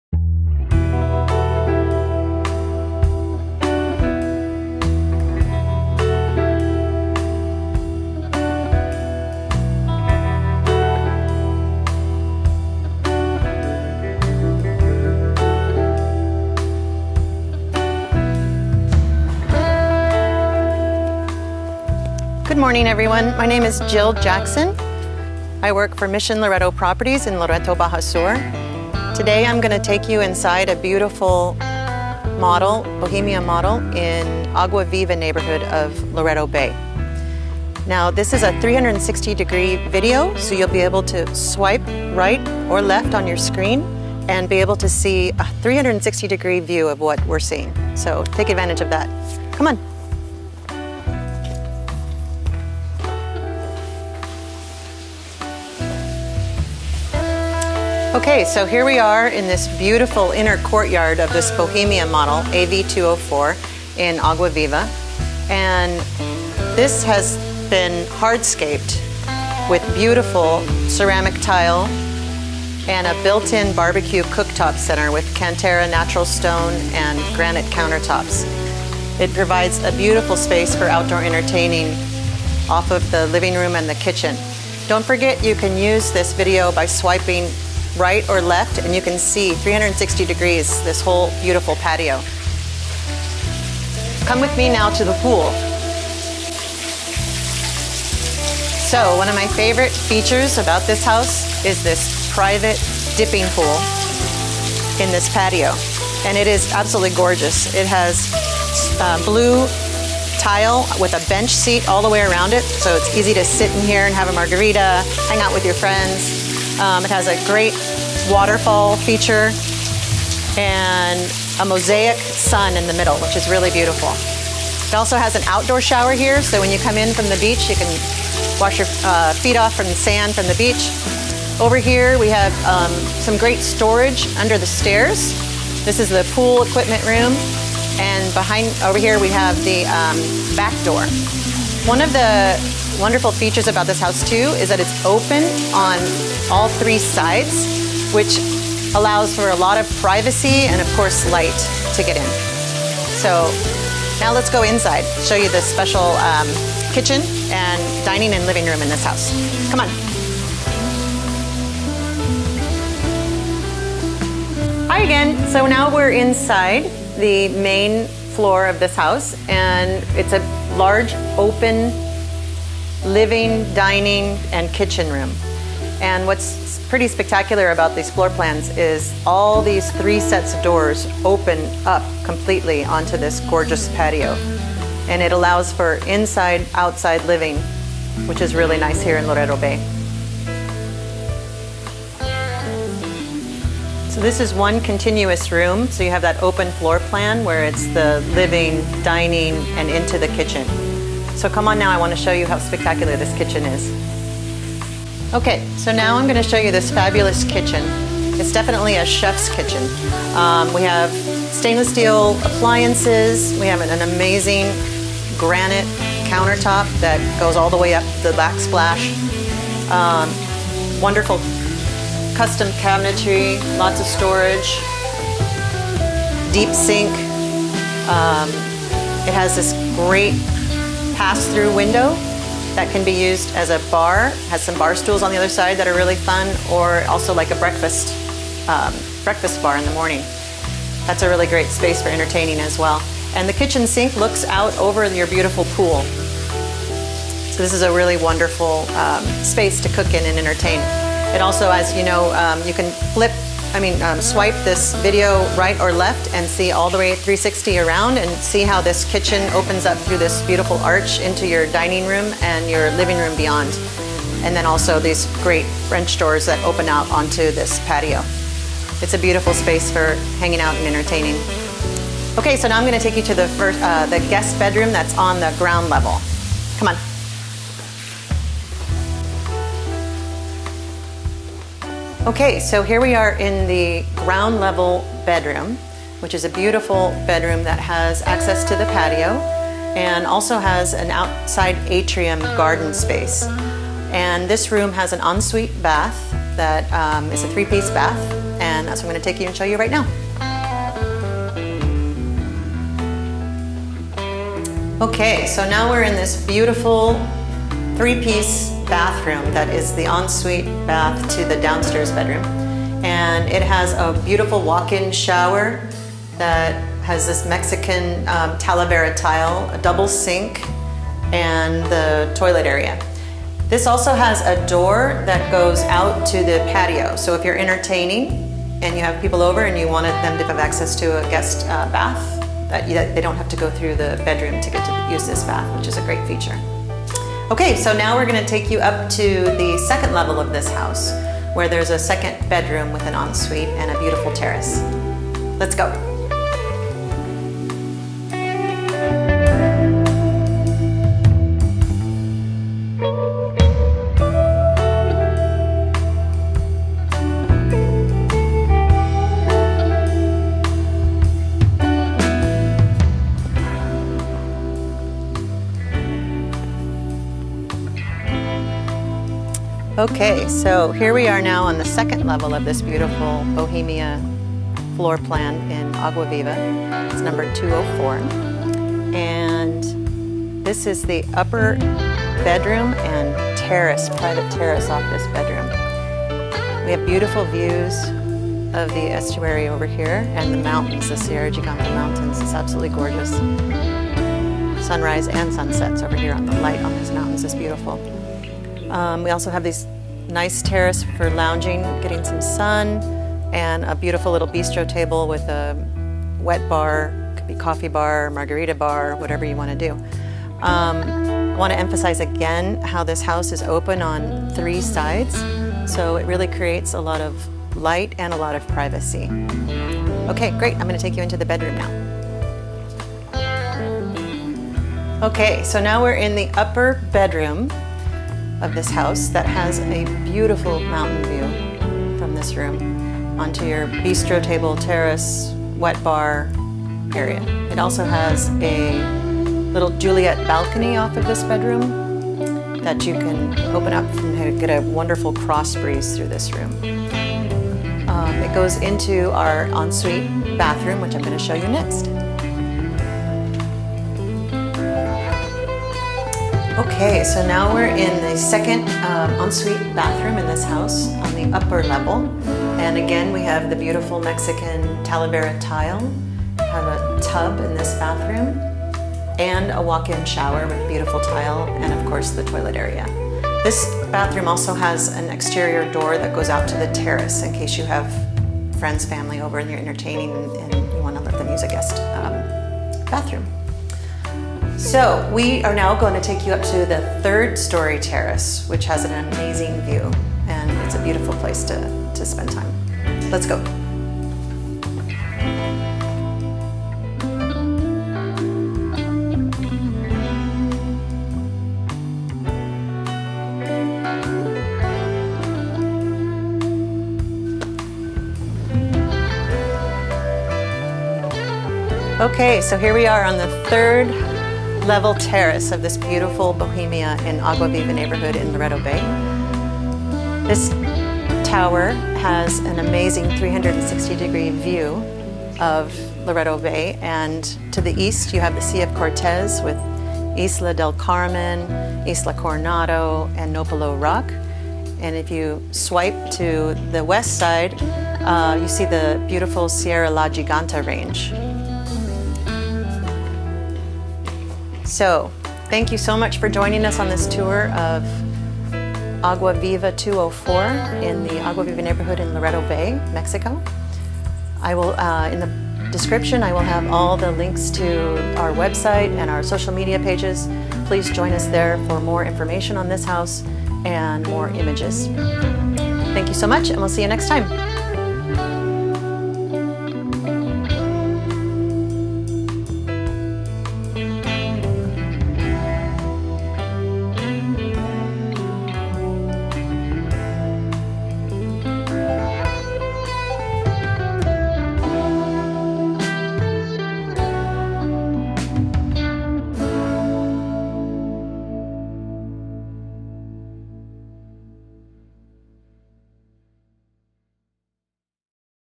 Two Bedroom/Two Bathroom Home with pool For Sale in Loreto Bay, Baja California Sur, Mexico. SOLD
360 degree video! (Please make sure you watch on the highest quality settings.) Don't miss this opportunity to own one of the nicest two bedroom two bathroom homes in Loreto Bay, providing the best in outdoor living, spectacular views and private pool!   This popular Bohemia floor plan is ideally located in the Agua Viva neighborhood of Loreto Bay, and is situated on a corner, (open on three sides), which allows for privacy and natural light and invites the outdoors in.   for more information and photos.  Link to the listing

This special home offers many opportunities for outdoor living!  Enter through the front door and into a large ceramic tiled courtyard complete with a built in gas BBQ and bar made of natural stone, and an entertaining area with comfortable outdoor furniture.    This  courtyard is larger than most, and wraps around the house where you will discover a private, beautifully tiled small pool with a custom tiled mosaic and a waterfall feature.   There is an outdoor shower for washing off the sand from the beach, and additional seating area in front of the pool that is covered by a pergola providing shade.

The entire first floor of the house opens out onto this gorgeous patio!
The open living and dining room has three sets of glass doors that open out onto this courtyard allowing for seamless indoor and outdoor living.   The gourmet kitchen is open to the dining room and framed by a beautiful archway and is complete with stainless steel appliances, dishwasher and stunning granite countertops.  There is a large window over the deep sink that looks out onto the private pool, and a fabulous pass through window that opens onto the courtyard that serves as a breakfast bar or a perfect place to enjoy a cocktail with friends.  The first floor bedroom opens out through glass doors onto the courtyard, and also has a set of double glass doors on the opposite side of the room which leads to a small private space open to the outdoors that allows the breeze to flow through the room.   This bedroom has a beautiful ensuite bath complete with a Talavera tiled shower and double sinks.  There is an additional door to leading to the courtyard from the bath, providing easy access for guests while entertaining.

Wonderful Mountain views from the second floor terrace and bedroom!
Make your way up the stairs to the second level where you will find a large terrace and breathtaking views of the Sierra la Giganta Mountains.  On this level there are lounges for sun bathing, a Talavera tiled wet bar, a bistro table and chairs, and the second bedroom with a three piece Talavera tiled ensuite bathroom.  (Tub and separate walk in shower).    This large bedroom has a set of glass doors that open onto the terrace with the views of the mountains beyond and another set of glass doors on the opposite side of the room with a Juliete balcony.

Wander up the colorfully tiled staircase to the third story terrace where you will find a bistro table for six and spectacular 360 degree views of the Sea, Islands and the Mountains!
This home is sold furnished with high quality furnishings and has been meticulously maintained by the current owners, and has just been painted.  Turn key home!

Just a few of the special features of this home;
Large outdoor living space with built in BBQ on ground level
Small private pool
Water softener and purification system (drink the water directly from the tap!)
Designated Golf Cart parking outside and space inside to park it while you are away
Third story tower with amazing water and mountain views
Located in a Very quiet and peaceful area of Loreto Bay
Enclosed storage under the stairs and in laundry closet

Loreto Bay is an active community located on the Sea of Cortes a few miles South of the historic town of Loreto, Baja Sur Mexico.  Loreto Bay has many amenities to enjoy; a golf course, a gorgeous beach (snorkel right off the beach!), restaurants and shops.  Loreto has an international airport with direct flights from Los Angeles, Calgary, Phoenix and Tijuana.  With the beautiful islands off shore and the Sierra la Giganta Mountains as a back drop, coupled with the friendly people and great food, Loreto is a very special destination and a wonderful place to live.